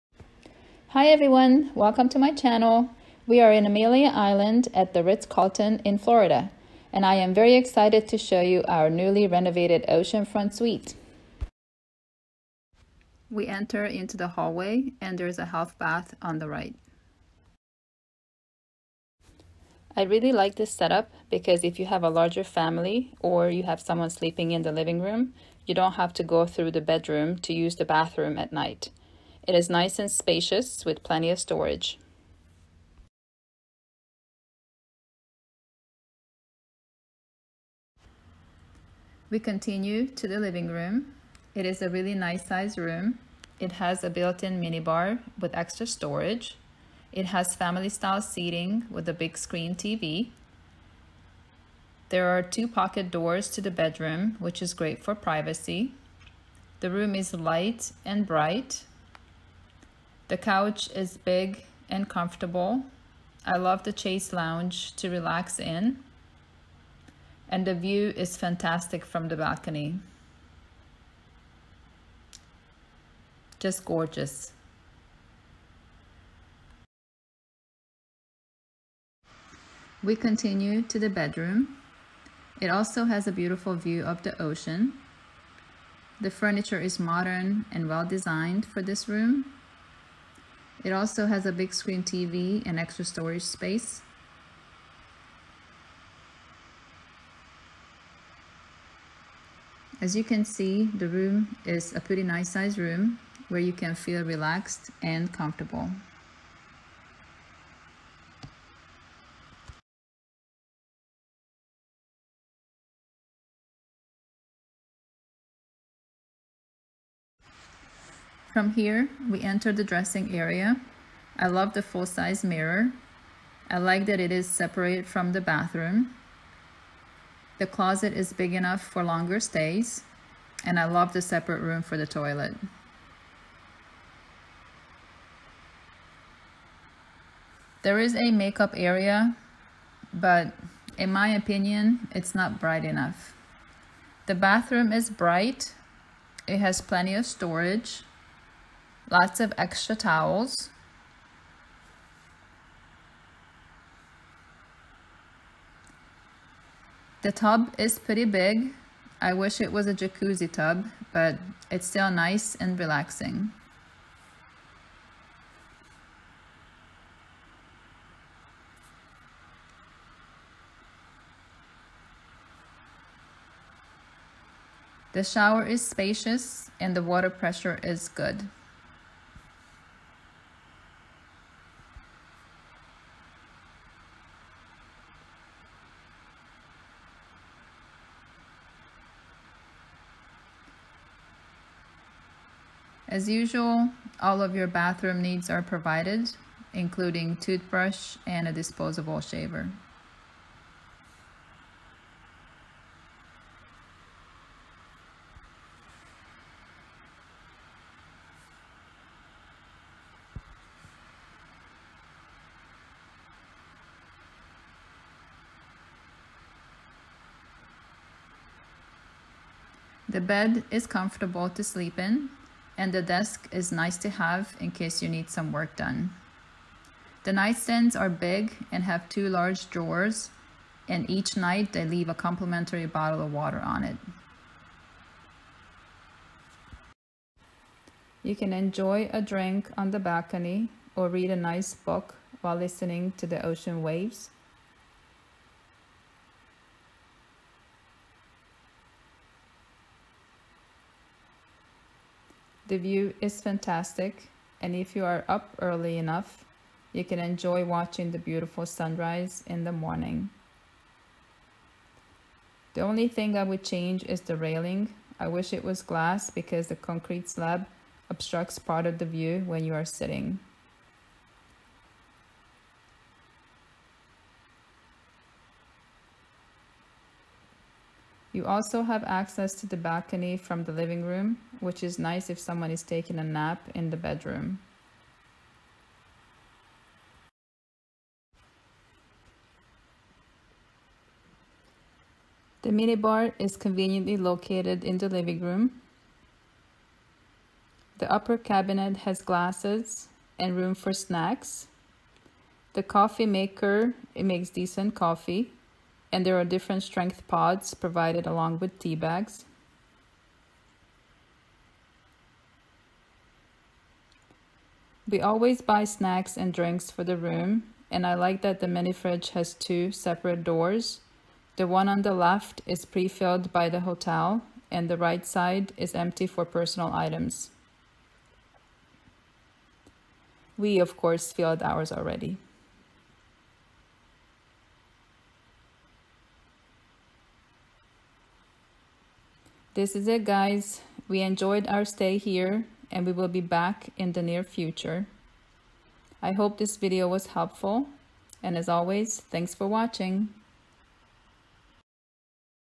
The Ritz Carlton Amelia Island Ocean Front Suite Tour (Newly Renovated!)
This video is a tour of one of the newly renovated suites at the Ritz Carlton Amelia Island Luxury Beach Resort